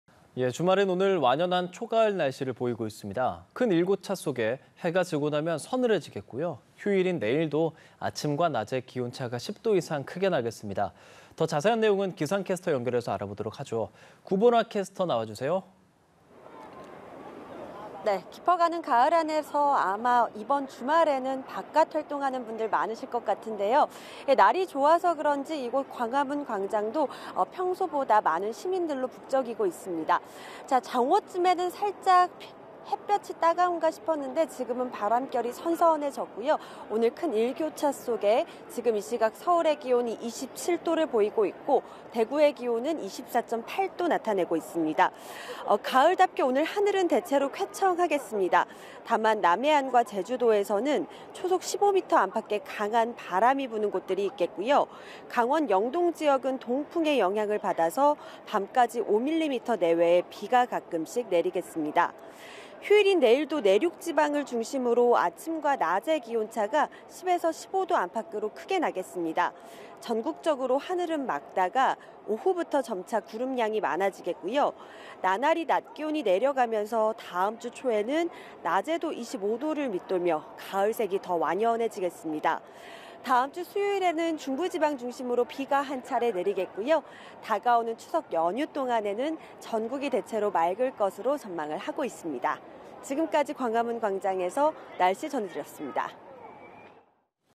[날씨] 주말 완연한 초가을…해 지면 서늘, 일교차 유의 / 연합뉴스TV (YonhapnewsTV)
[날씨] 주말 완연한 초가을…해 지면 서늘, 일교차 유의

[앵커]

주말인 오늘 완연한 초가을 날씨를 보이고 있습니다.

큰 일교차 속에, 해가 지고 나면 서늘해지겠고요.

휴일인 내일도 아침과 낮의 기온차가 10도 이상 크게 나겠습니다.

더 자세한 내용은 기상캐스터 연결해서 알아보도록 하죠.

구본아 캐스터 나와주세요.

[캐스터]

깊어가는 가을 안에서 아마 이번 주말에는 바깥 활동하는 분들 많으실 것 같은데요.

날이 좋아서 그런지 이곳 광화문 광장도 평소보다 많은 시민들로 북적이고 있습니다.

정오쯤에는 살짝 햇볕이 따가운가 싶었는데 지금은 바람결이 선선해졌고요.

오늘 큰 일교차 속에 지금 이 시각 서울의 기온이 27도를 보이고 있고, 대구의 기온은 24.8도 나타내고 있습니다.

가을답게 오늘 하늘은 대체로 쾌청하겠습니다.

다만, 남해안과 제주에서는 초속 15m 안팎의 강한 바람이 부는 곳이 있겠고요.

강원영동지역은 동풍의 영향으로 밤까지 5mm 내외의 비가 가끔씩 내리겠습니다.

휴일인 내일도 내륙을 중심으로 아침과 낮의 기온차가 10에서 15도 안팎으로 크겠습니다.

전국적으로 하늘은 맑다가 오후부터 점차 구름 양이 낮아지겠고요.

나날이 낮 기온이 내려가면서 다음 주 초에는 낮에도 25도를 밑돌며 가을색이 더 완연해지겠습니다.

다음 주 수요일에는 중부지방 중심으로 비가 한 차례 내리겠고요.

다가오는 추석 연휴 동안에는 전국이 대체로 맑을 것으로 전망을 하고 있습니다.

지금까지 광화문 광장에서 날씨 전해드렸습니다

(구본아 기상캐스터)